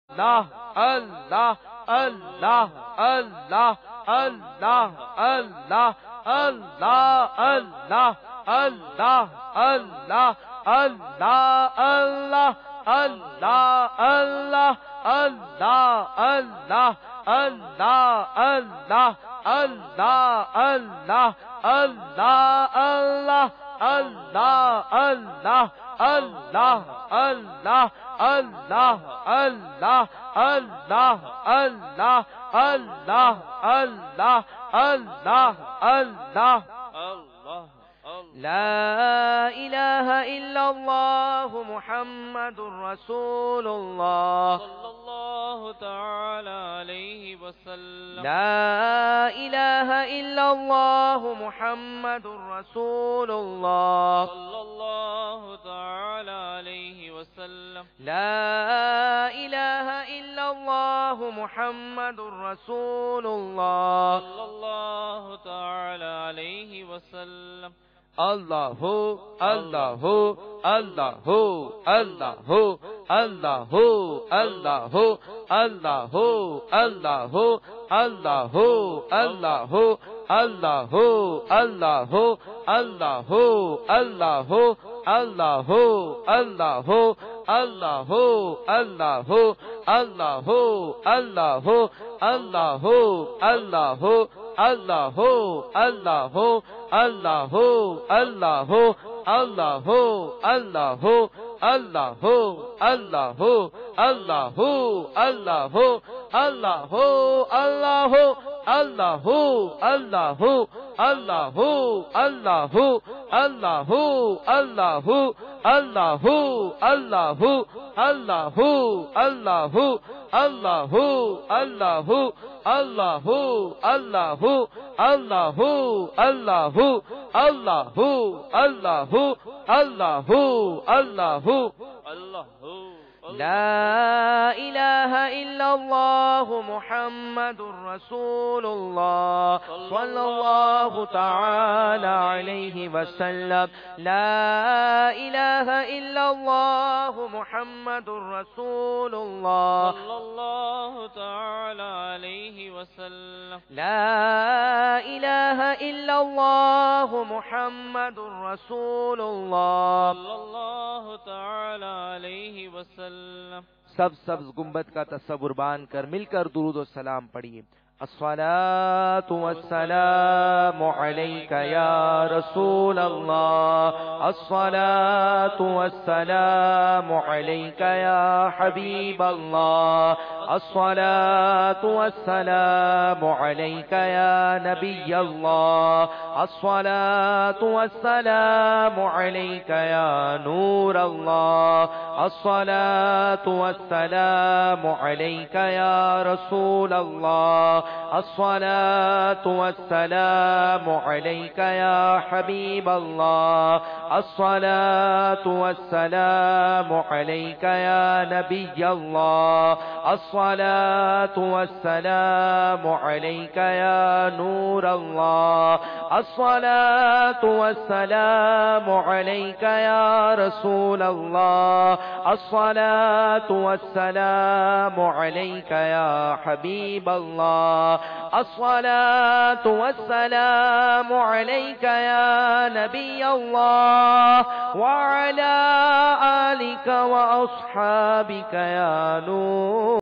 DawateIslami Ijtima - Zikr e Elahi - Madani Channel Video
Islamic Brothers are doing Zikr Allah in Ijtima e Aam (DawateIslami Ijtima) at Faizan e Madina - Global Islamic Centre (Aalami Madani Markaz).
The Viewers can watch Madani Channel streaming on Internet to visit this Link i.e.